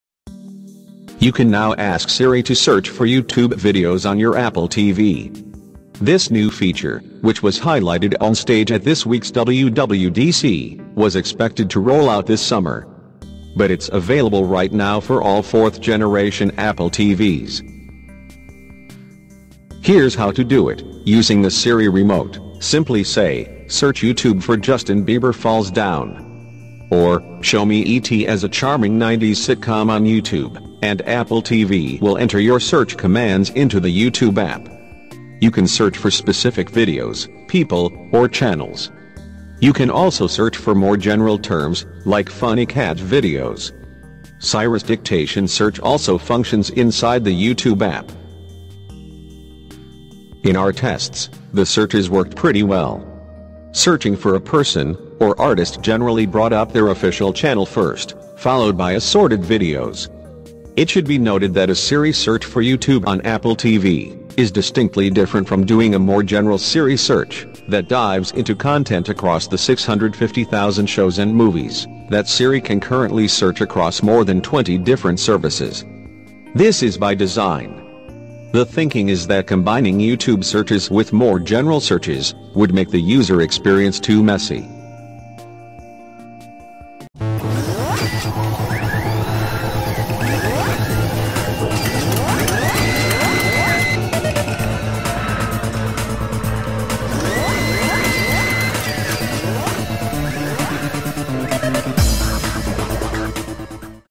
How to use Siri to search YouTube on your Apple TV
You can now ask Siri to search for YouTube videos on your Apple TV.

This new feature, which was highlighted onstage at this week's WWDC, was expected to roll out this summer. But it's available right now for all fourth-generation Apple TVs.